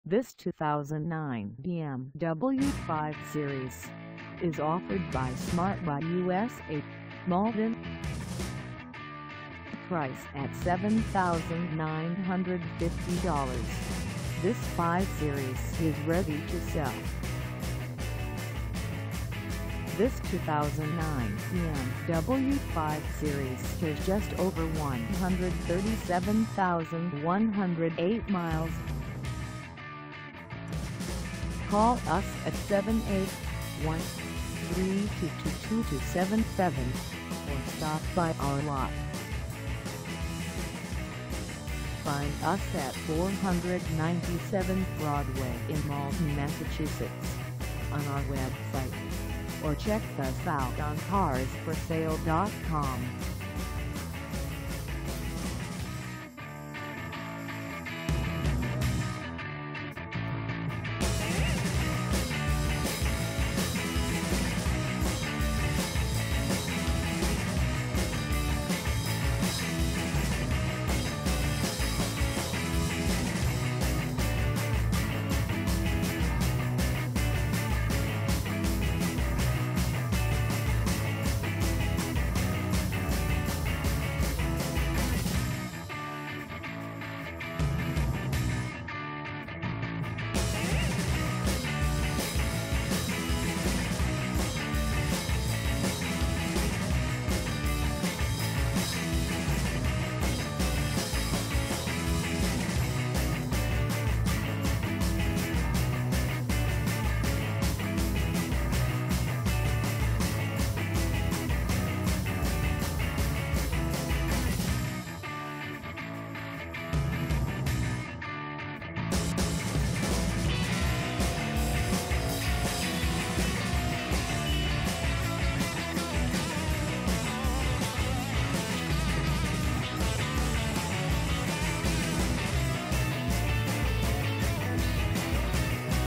2009 BMW 5 Series 528i xDrive AWD 4dr Sedan for sale in MALD
This 2009 BMW 5 Series 528i xDrive AWD 4dr Sedan is for sale in MALDEN, MA 02148 at Smart Buy USA - Malden.

Engine: 3.0L I6
Transmission: Automatic 6-Speed
Color: Unspecified
Mileage: 137108
Contact Smart Buy USA - Malden 497 Broadway MALDEN, MA 02148 to test drive this 2009 BMW 5 Series 528i xDrive AWD 4dr Sedan today.